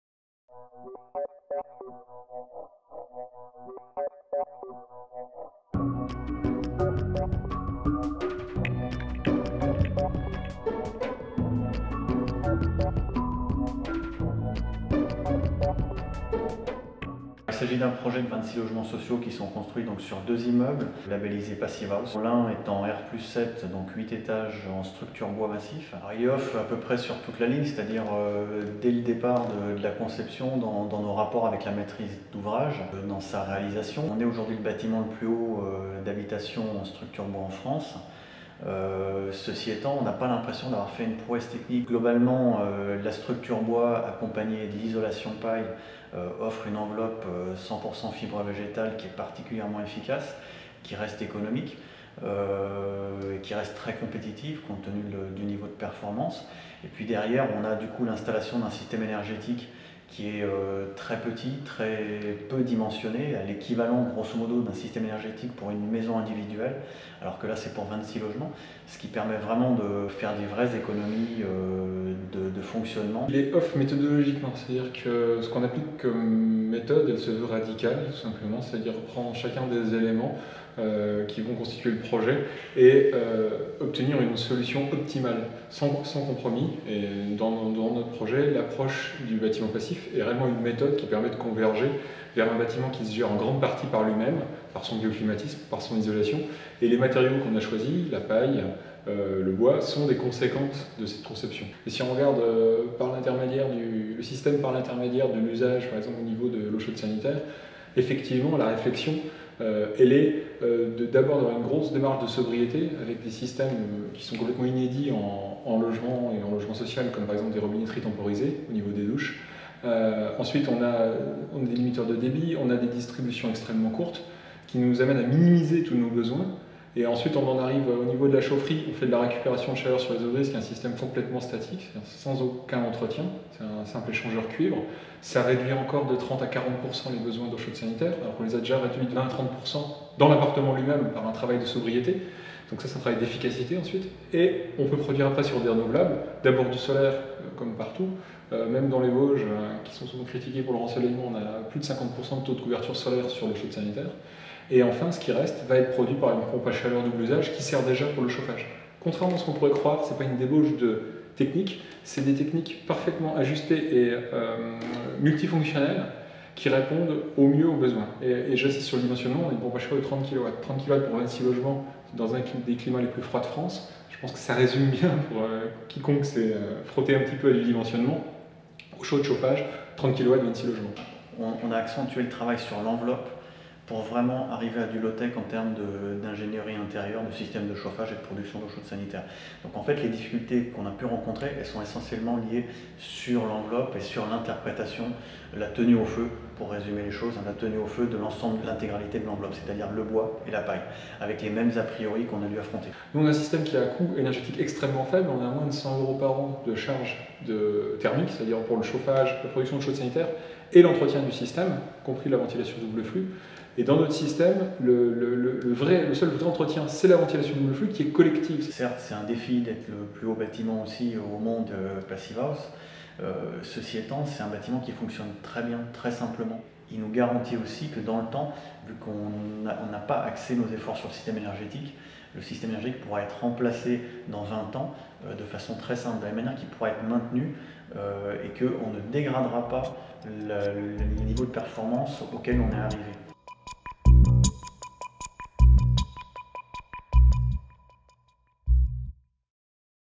LeOffduDD2015 - Residence Jules Ferry, Saint Dié des Vosges (88)
Construction de 28 logements sociaux passifs en panneaux de bois massif CLT et isolation en paille, dont un immeuble R+7.
ASP Architecture
Maîtrise d'ouvrage : Le Toit Vosgien